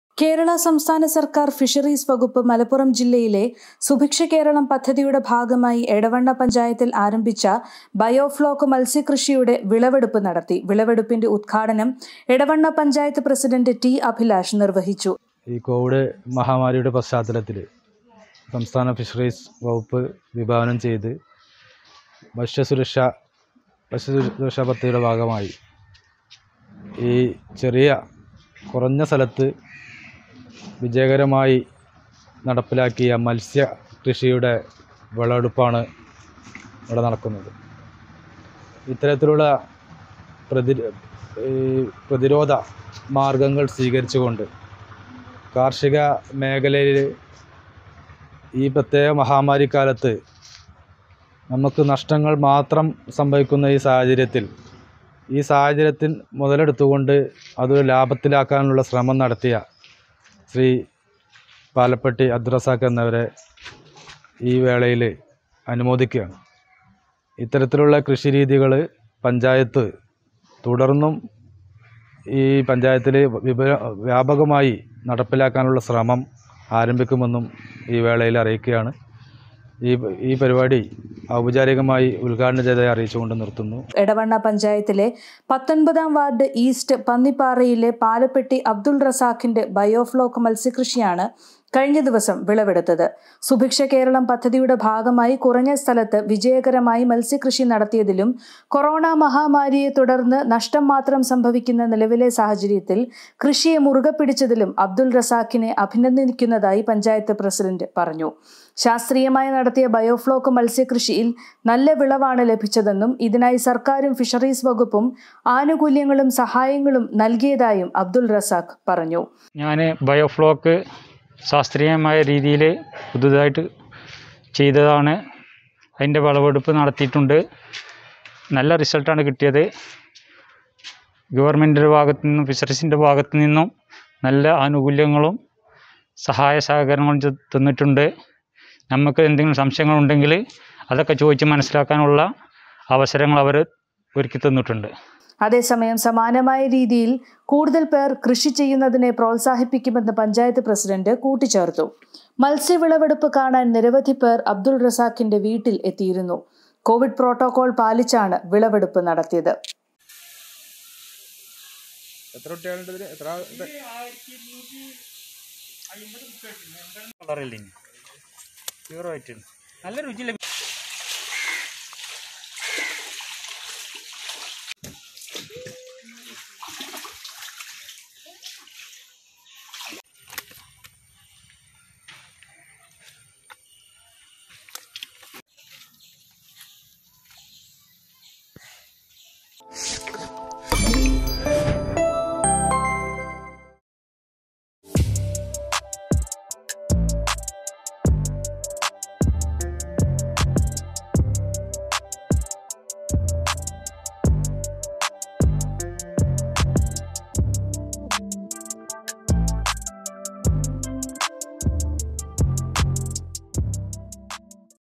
കുറഞ്ഞ ചെലവിൽ മത്സ്യകൃഷി ബയോഫ്ലോക് ആദായകരമെന്ന് അബ്ദുൾ റസാഖ് | Biofloc Fish Farming
കേരള സംസ്ഥാന സർക്കാർ ഫിഷറീസ് വകുപ്പ് മലപ്പുറം ജില്ലയിലെ സുഭിക്ഷ കേരളം പദ്ധതിയുടെ ഭാഗമായി എടവണ്ണ പഞ്ചായത്തിൽ ആരംഭിച്ച ബയോഫ്ലോക് മത്സ്യ കൃഷിയുടെ വിളവെടുപ്പ് നടത്തി. വിളവെടുപ്പിന്‍റെ ഉദ്ഘാടനം എടവണ്ണ പഞ്ചായത്ത് പ്രസിഡന്‍റ്  ടി. അഭിലാഷ് നിർവഹിച്ചു.
എടവണ്ണ പഞ്ചായത്തിലെ പത്തൊമ്പതാം വാർഡ് ഈസ്റ്റ് പന്നിപ്പാറയിലെ പാലപ്പെട്ടി അബ്ദുൾ റസാഖിന്‍റെ ബയോ ഫ്ലോക്ക് മത്സ്യകൃഷിയാണ് കഴിഞ്ഞ ദിവസം വിളവെടുത്തത്